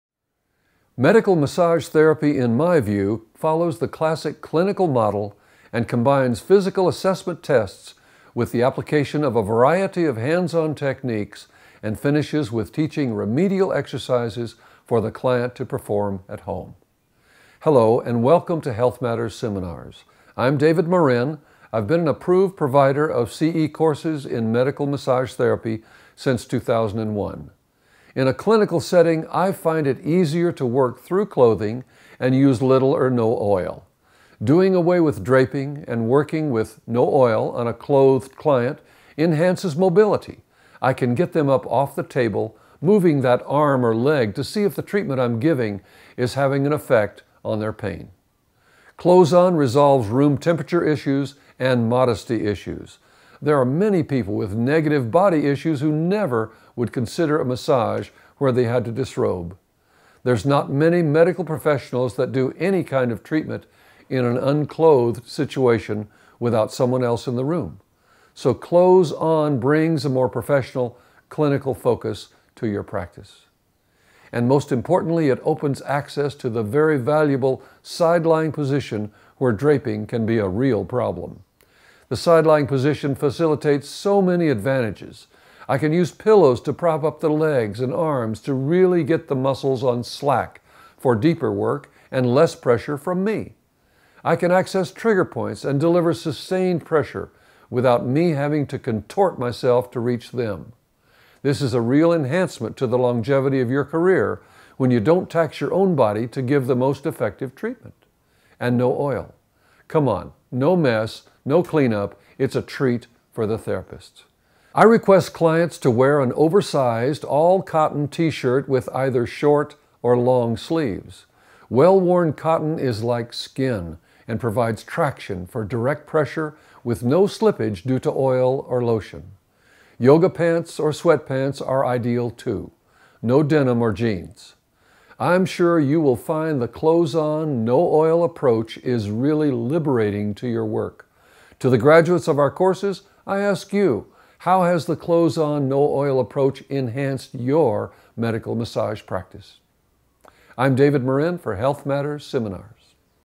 David Morin Medical Massage Therapy Clothes-on, No Oil Approach.
David Morin explains how the Clothes on, No Oil approach to Medical Massage Therapy is appropriate for massage in a Clinical setting. It solves modesty issues and opens up the valuable side lying position for superior treatment.

Medical Massage Therapy in my view follows the classic clinical model and combines physical assessment tests with the application of a variety of hands-on techniques, and finishes with teaching remedial exercises for the client to perform at home.

Hello and Welcome to Health Matters Seminars
I’m David Morin. I’ve been an Approved Provider of CE Courses in Medical Massage Therapy since 2001.

In a clinical setting, I find it easier to work through clothing and use little or no oil. Doing away with draping and working with No-Oil on a Clothed Client enhances mobility, I can easily get them up off the table, moving that arm or leg to see if the treatment I’m giving is having an effect on their pain.

Clothes on resolves room temperature issues and modesty issues. There are many people with negative body issues who would never consider a massage where they had to disrobe. There’s not many medical professionals that do any kind of treatment in an unclothed situation without someone else in the room, so clothes-on brings a more professional, clinical focus to your practice.

And most importantly it opens access to the very valuable side-lying position where draping can be a real problem. The sidelying position facilitates so many advantages. I can use pillows to prop up legs and arms to really get the muscles on slack for deeper work with less pressure from me.

I can access trigger points and deliver sustained pressure without me having to contort myself to reach them. This is a real enhancement to the longevity to your career when you don’t tax your own body to give the most effective treatment.

And no oil. Come on, no mess, no clean up, it’s a treat for the therapist.

I request clients to wear an oversized all cotton t-shirt with either short or long sleeves. Well worn cotton is like skin and provides traction for direct pressure with no slippage due to oil or lotion. Yoga pants or sweat pants are ideal too. No denim

I’m sure you’ll find the clothes-on no oil approach is really liberating to your work. To the graduates of our courses, I ask you, how has the clothes-on, no-oil approach enhanced your Medical Massage Practice?

I'm David Morin for Health Matter Seminars.